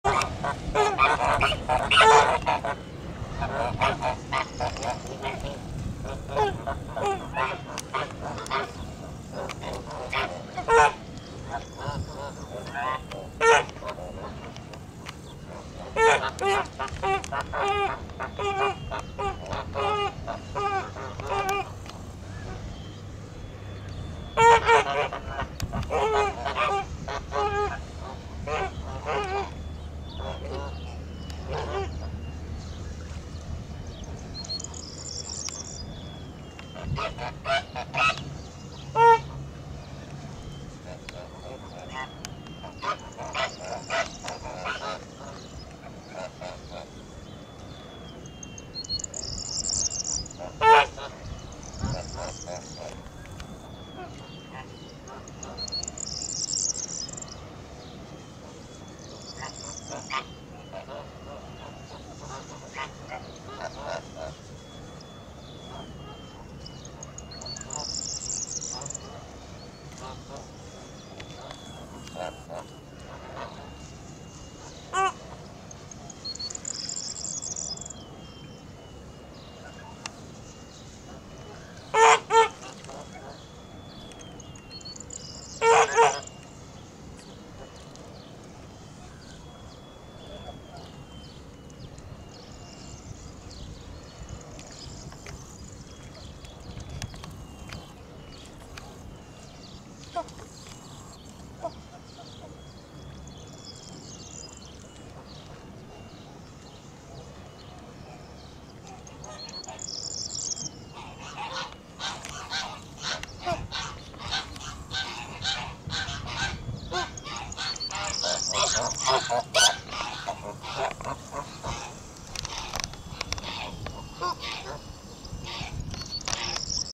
24.04.2018.Video2.After a breeding period.OLYMPUS farm.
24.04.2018. Video 2. After a breeding period. Whoever no longer gives eggs and does not guard a lying female is at large. OLYMPUS farm.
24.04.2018.След размножителен период.  Който вече не дават яйца и не пазят мътещи гъски са на свобода. Ферма Олимп.